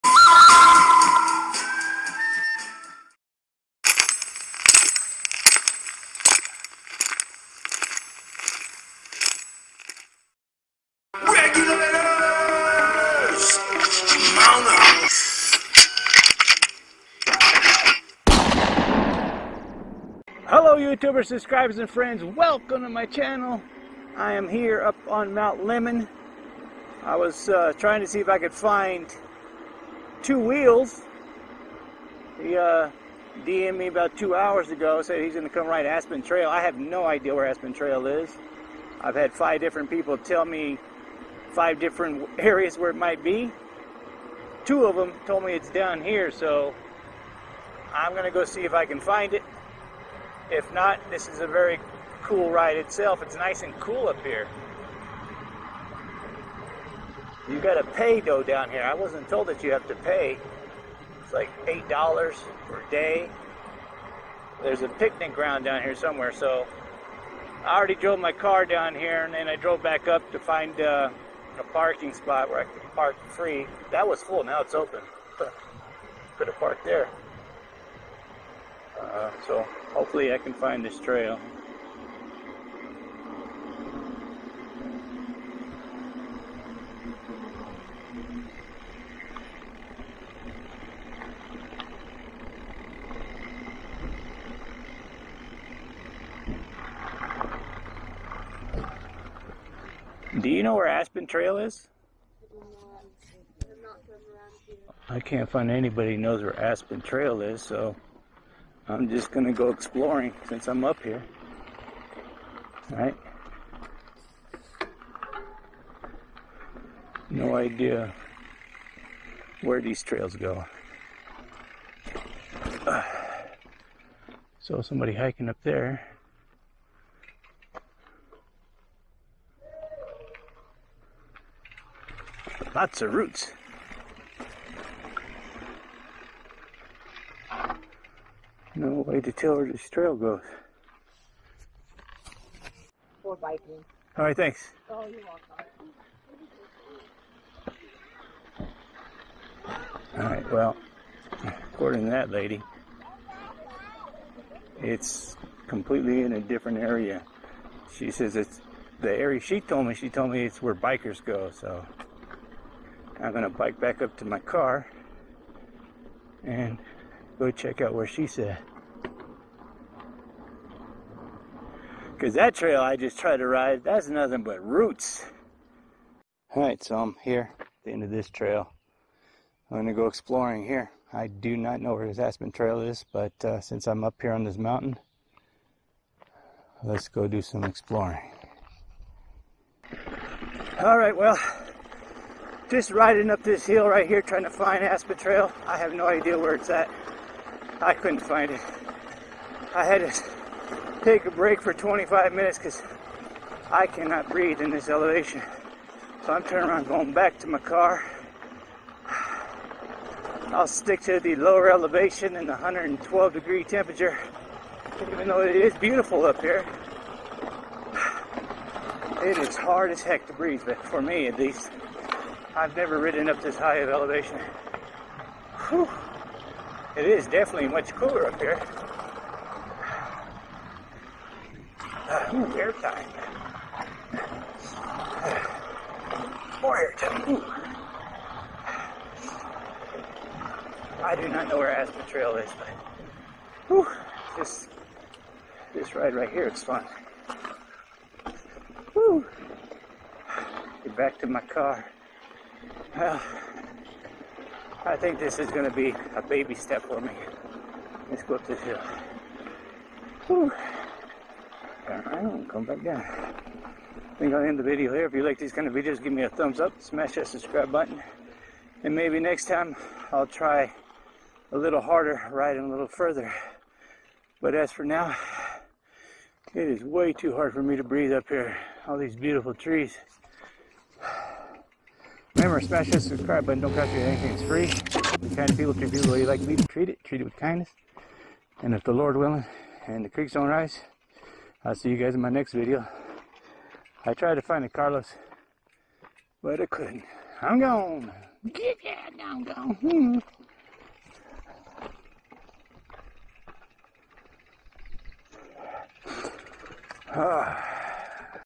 Trying To Ride MTB At 9000 Foot Elevation
Riding Mountain Bikes and Learning More about Riding.